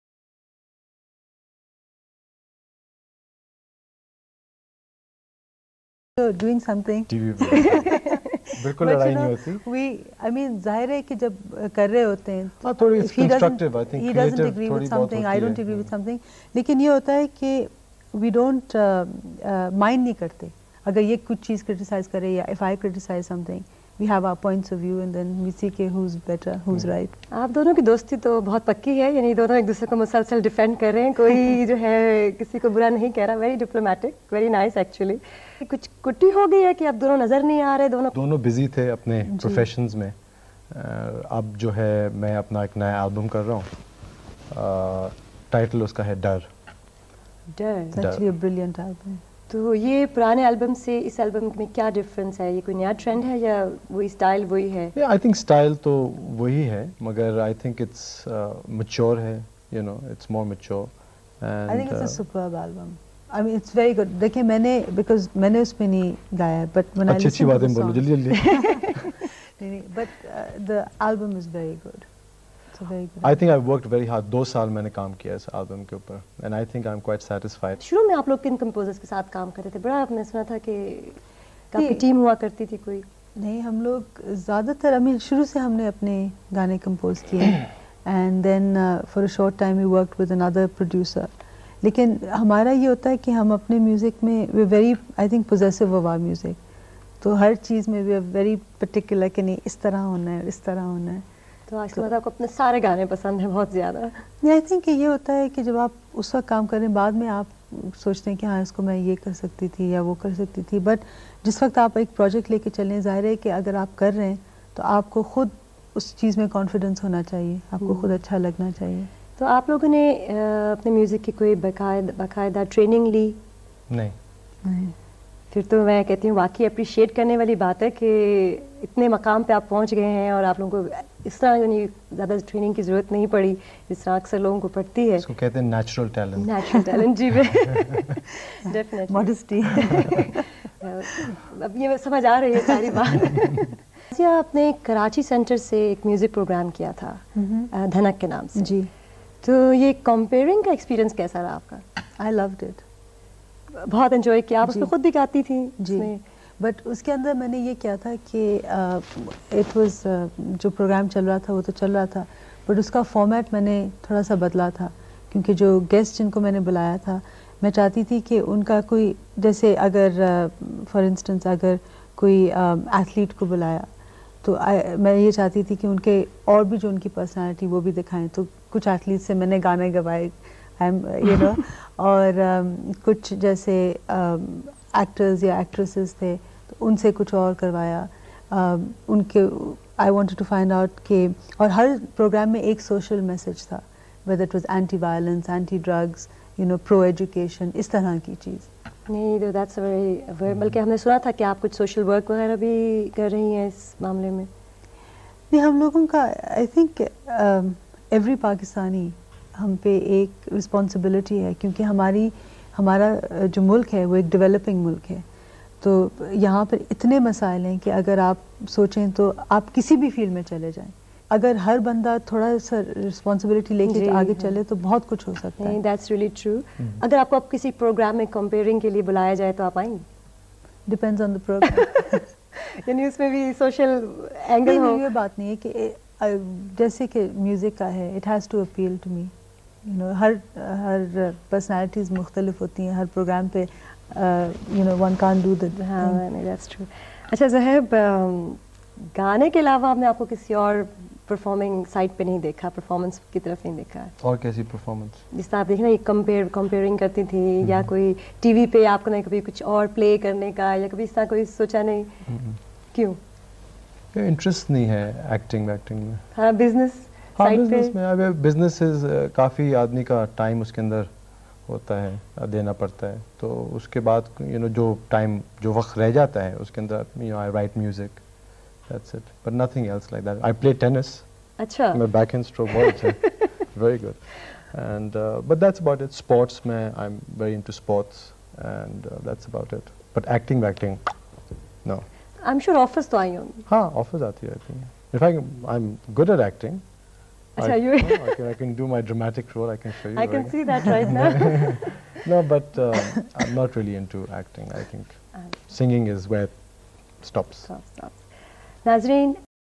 Nazia Hassan talking about her life secrets and ambitions | Complete Interview
Nazia Hassan and Zohaib Hassan Interview talking about their albums, TV hosting experience and more.